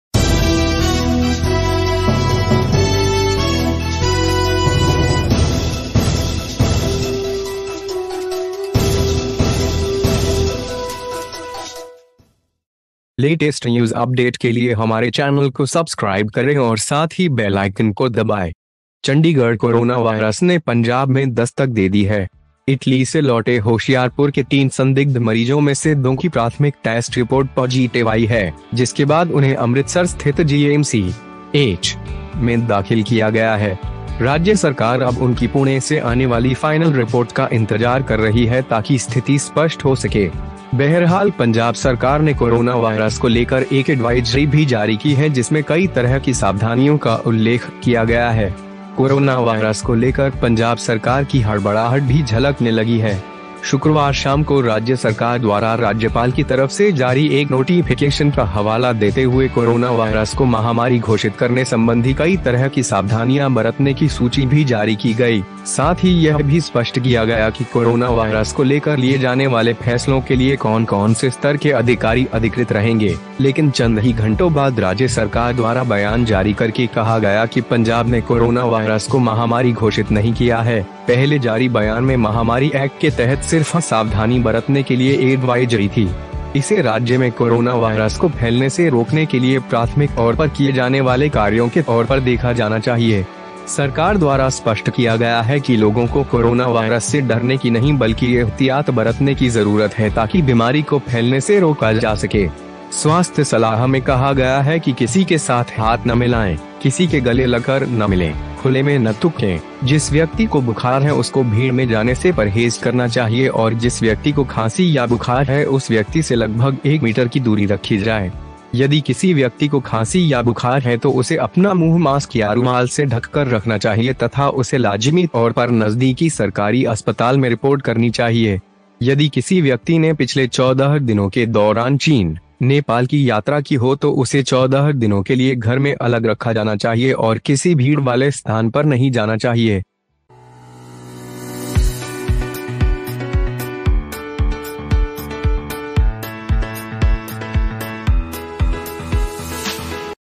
पंजाब में Corona वायरस की दस्तक, प्राथमिक रिपोर्ट में होशियारपुर के 2 मरीज पॉजीटिव
Home Punjab Corona virus knock, in primary report, 2 patients of Hoshiarpur, Positivechandigarh Corona virus have knocked in Punjab. Of the 3 suspected patients from Hoshiarpur who returned from Italy, the primary test report of 2 has come up positive, after which they were called GMC in Amritsar. Has been filed in H.K. The state government is now waiting for their final report from Pune to clarify the situation. However, the Punjab government has also issued an advisory regarding corona virus in which several precautions have been mentioned. The Punjab government panic over the Corona virus is also beginning to be seen. On Friday evening, the state government issued a list of precautions to be taken to declare the corona virus an epidemic, citing a notification issued by the governor, as well as clarifying that the corona virus should be taken. What level of officials will be authorized for decisions, but after a few hours, the state government issued a statement saying that Punjab has given the core The virus has not been declared a pandemic, with the advisory being issued only as a precaution under the pandemic act in an earlier statement. This should be seen as the primary action to be taken to prevent the spread of corona virus in the state. It has been clarified by the government that people need to take precaution not to fear the corona virus but to prevent the spread of the disease. Health advice states that do not join hands with anyone, do not embrace anyone, do not spit in the open, the person who has a fever should avoid going into the crowd and the person who has a cough or fever Keep a distance of about 1 meter. If a person has a cough or fever, he should cover his mouth with a mask or handkerchief and must report it to the nearest government hospital. If a person has traveled to China, Nepal during the last 14 days, then he should be kept indoors for 14 days and should not go to any crowded place.

VR NEWS !!